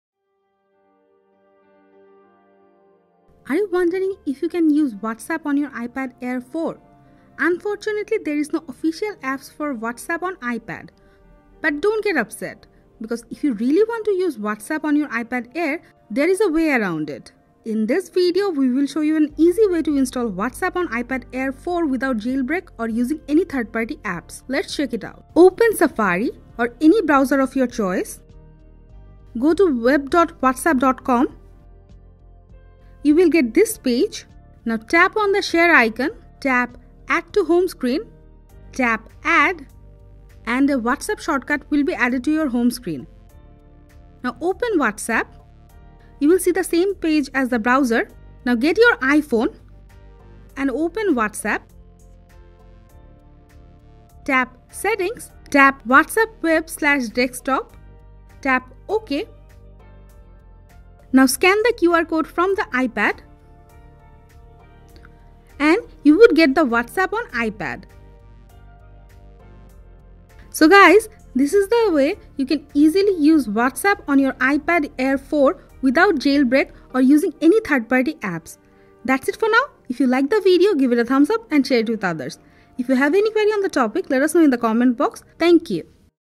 Install WhatsApp on iPad Air 4 (Without Jailbreak)
Are you wondering if you can use WhatsApp on your iPad Air 4? In this video we will show you step by step process to install WhatsApp on iPad Air 4 without jailbreak or any third party apps.

Unfortunately there is no official apps for WhatsApp on iPad. But don’t get upset, because if you really want to use WhatsApp on iPad Air, there are some ways around it and the best part is you won't have to use any third party apps.

Let's check out the steps you have to follow to use WhatsApp on iPad Air 4